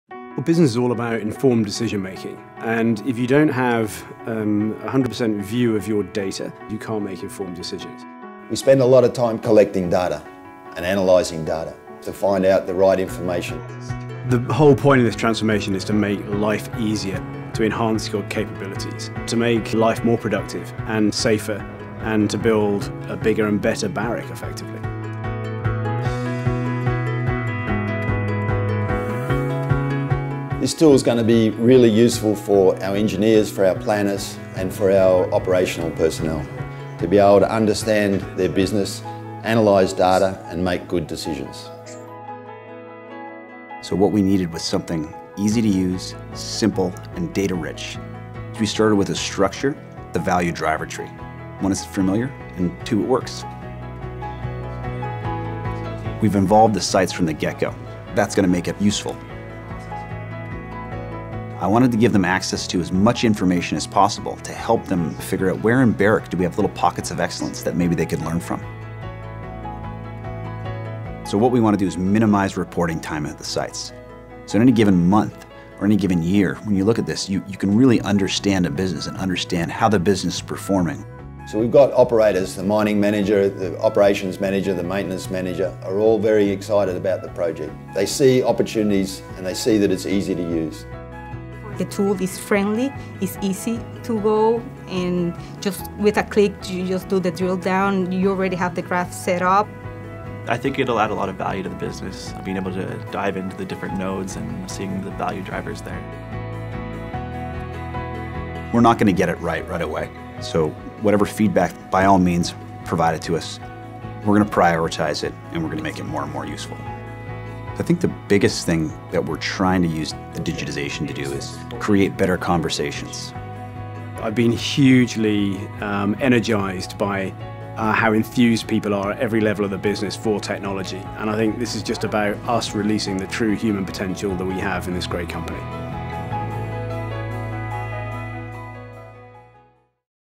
Digging deep for data
Gold isn’t the only thing we’re mining these days. In this video, see how our people developed an integrated planning tool that will mine millions of data points—helping our planners save time, and make better decisions. Good thing it comes with an unlimited data plan!